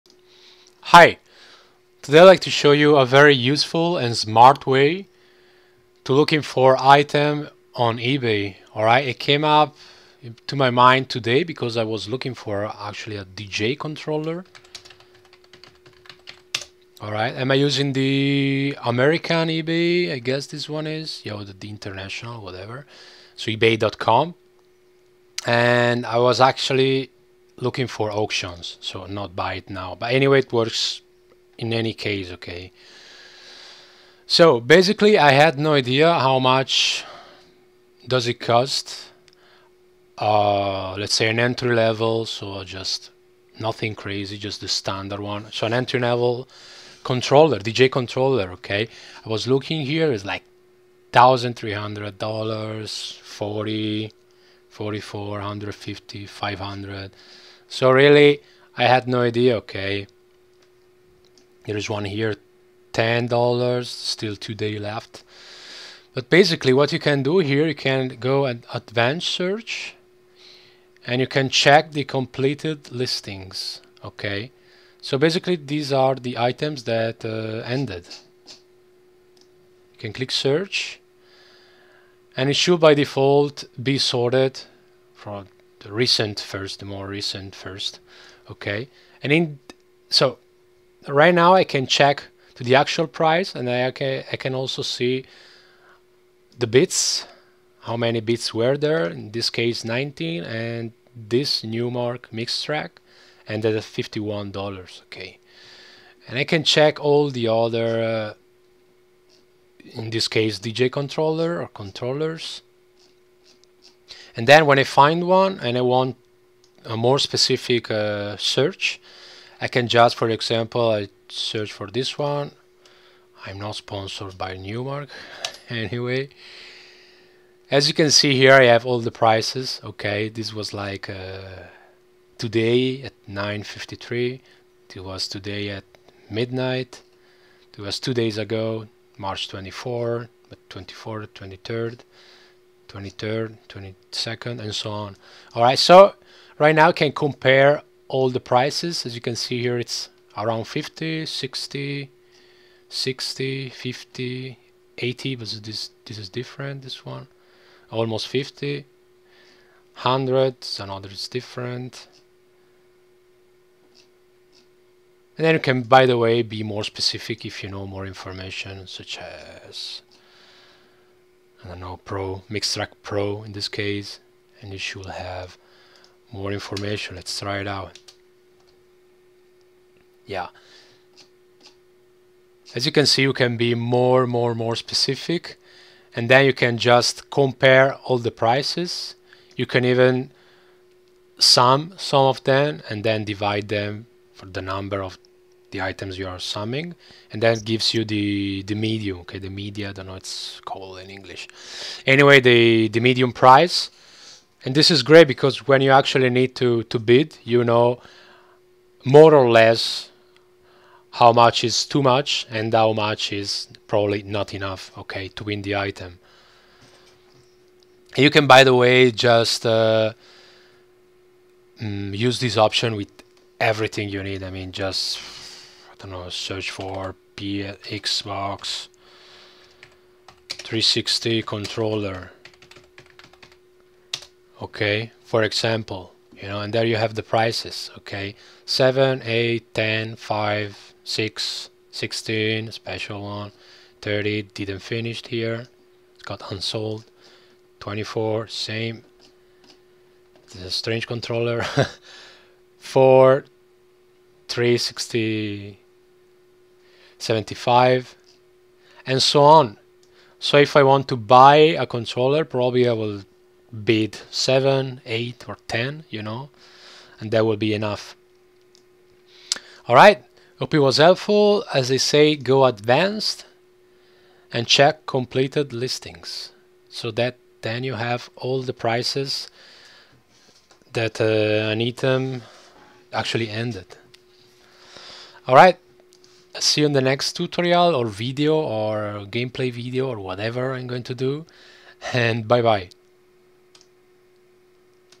Ebay ·· Smartly check how much an item should end up costing (Advance Search)
16GB Ram DDR3-1600 cl9
ASRock Fatal1ty p67 Performance
Shitty HDD